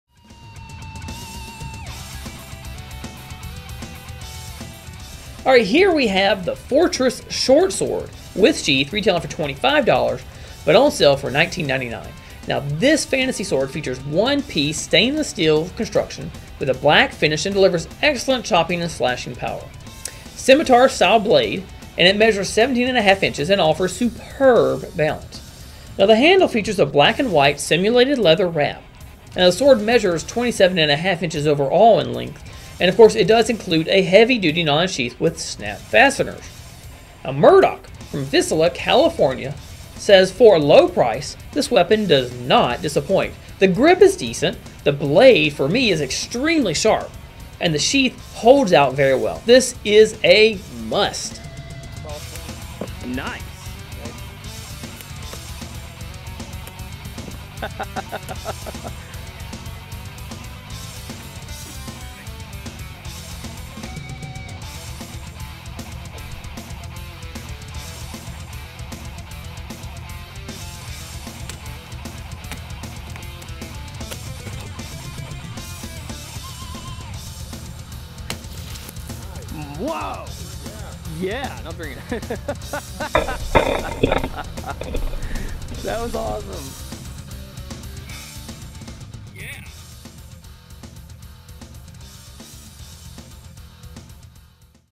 Fortress Short Sword with Sheath
This fantasy sword features one-piece stainless steel construction with a black finish and delivers excellent chopping and slashing power! The scimitar-style blade measures 17 1/2"" and offers superb balance. The handle features a black-and-white simulated leather wrap. This sword measures 27 1/2"" overall in length and includes a heavy duty nylon sheath with snap fasteners.